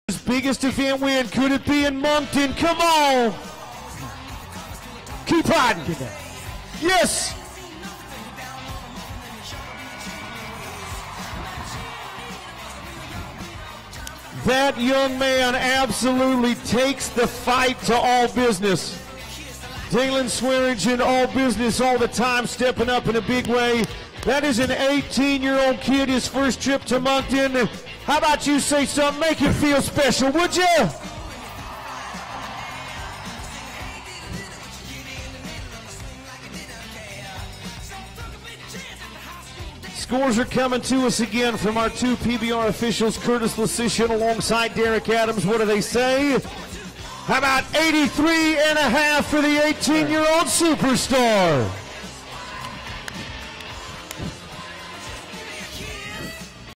Daylon Swearingen rides All Business for 83.5 points (PBR)
at stop No. 5 on the Monster Energy Tour in Moncton, New Brunswick at the Moncton Coliseum on June 2nd, 2018.